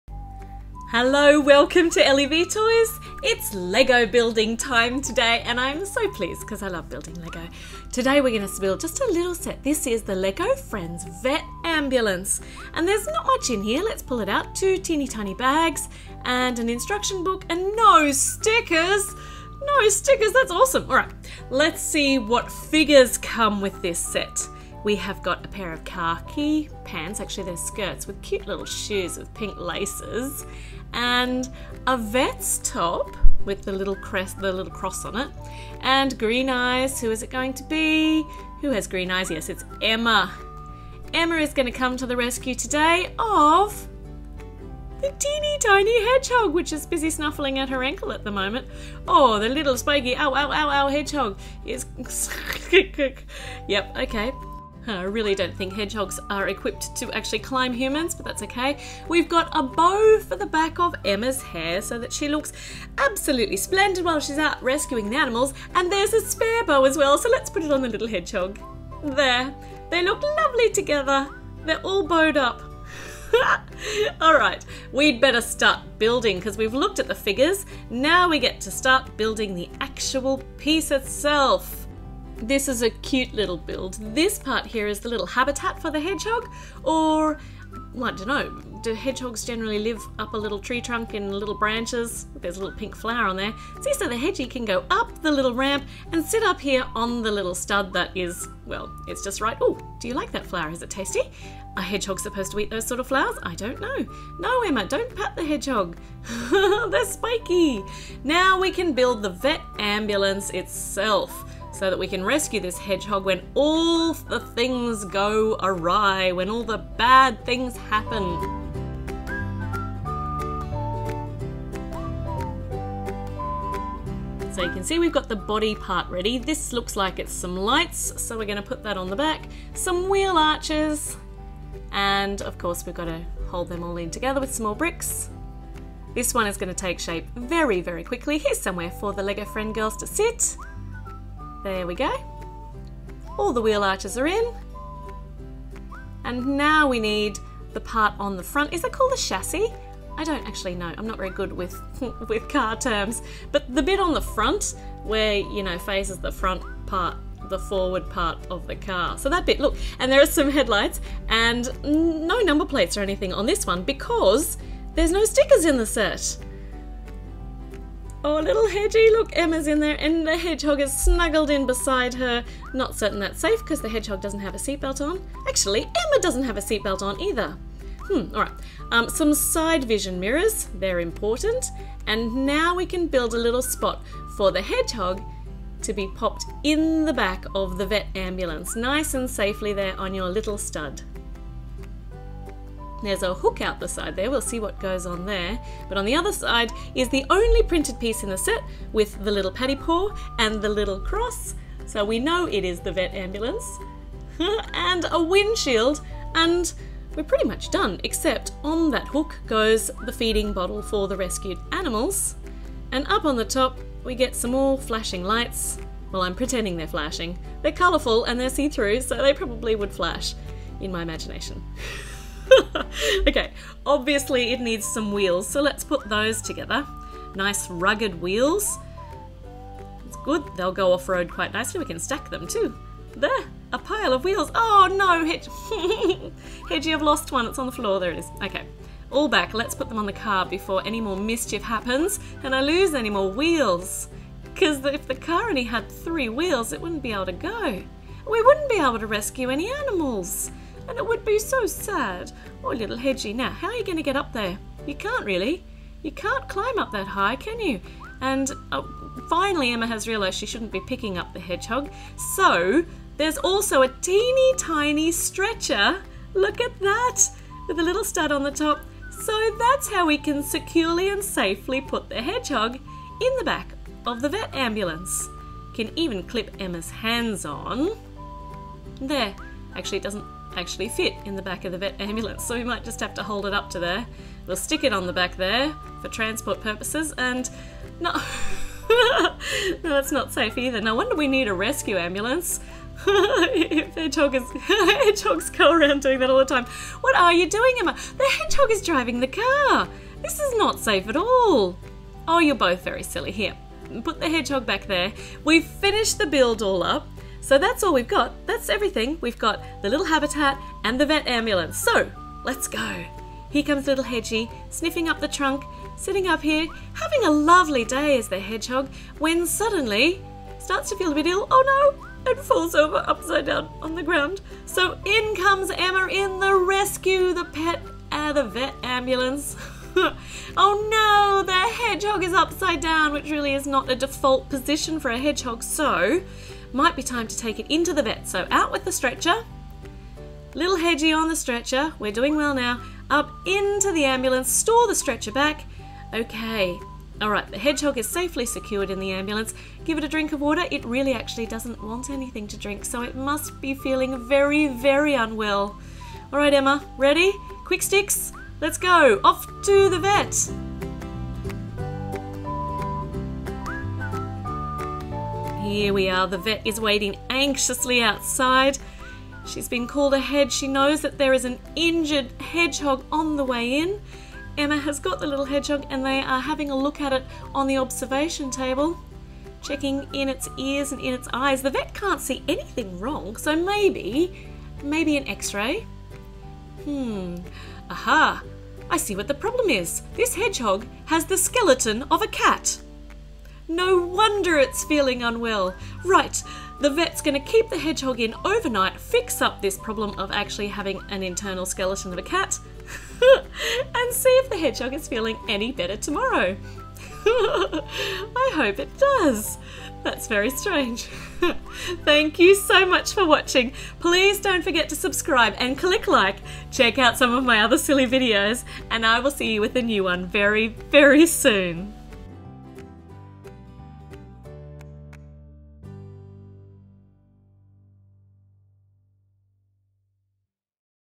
Lego Friends Vet Ambulance Hurt Hedgehog Rescue Build Silly Play - Kids Toys
Join Emma while she zooms around the countryside, rescuing hurt animals in her vet ambulance! The Lego Friends Vet Ambulance 41086 is a little set but full of play opportunities with minidoll Emma in her pet rescue uniform and the cutest little hedgehog. We'll take the poorly hedgehog to the Vet Clinic where an xray will reveal a hidden secret. 😉

Come to the rescue of injured animals in the Vet Ambulance! Jump into the Vet Ambulance and keep a lookout for animals in need! A little hedgehog playing in a bush has fallen from a branch. Help Emma speed to the rescue and check the hedgehog over. Give the hedgehog a drink from the feeding bottle and put him onto the stretcher to carry him to the Vet Ambulance. Combine with the 41085 Vet Clinic to give the hedgehog a full health check.